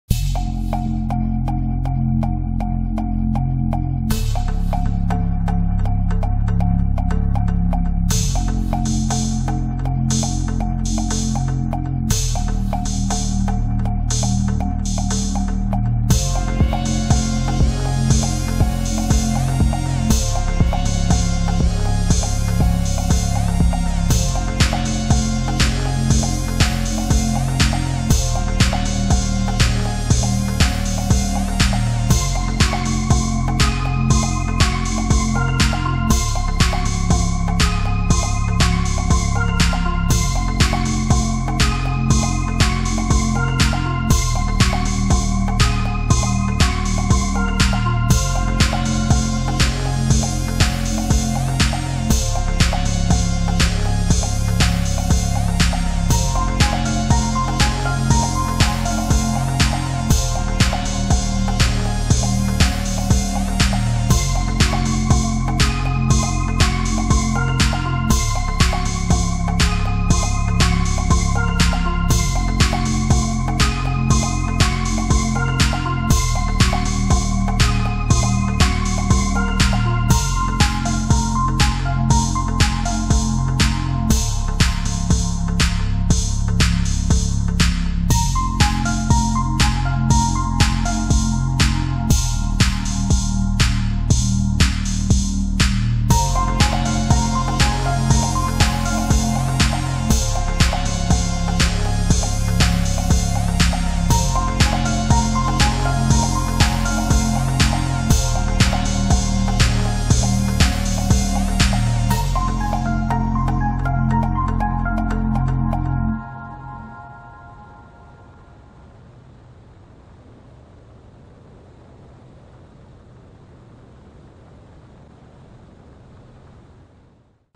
Voe comigo - Ilhabela em um avião monomotor !
Apenas uma pequena amostra do que vem por aí no video completo de um dos voos mais bonitos que já fiz...